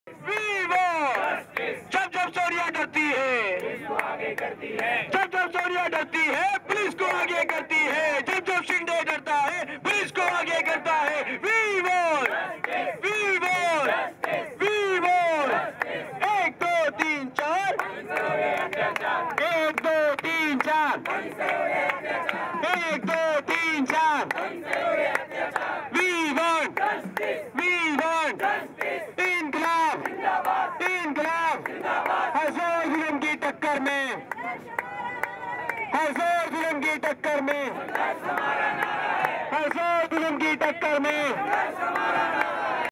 Delhi Police barriers hold back protestors
People are coming out to protest against the recent brutal gang-rape of a 23 year old girl that shook the entire nation. She was accompanied by a male friend after who was also viciously attacked and thrown off the bus. The budding physiotherapist was manhandled in a moving bus and later thrown off after stripping her naked on Sunday, 16th December 2012. The incident took place on a private chartered bus on the busiest road; Ring road in THE CAPITAL CITY OF INDIA, Delhi! The city which is also infamously termed as the Rape capital of India.

The condition of the girl remains critical yet continues to battle for her life. Doctors at Safdarjung Hospital where she is being treated says they've never seen a horrific case as this. The doctors are battling the onset of septicaemia - a systemic infection, usually caused by bacteria of various types contaminating a person's blood. In the raped girl's case, Septicemia may have been caused due to the infected cuts that she was inflicted with. Her small intestines had to be removed and even if she survives she may never eat again or live like a normal person.

According to statistics, a woman is raped every 18 hours or molested every 14 hours in the Capital. And more shockingly, the majority of the attackers are below 25 years. This year alone, some 600 cases have been recorded in New Delhi.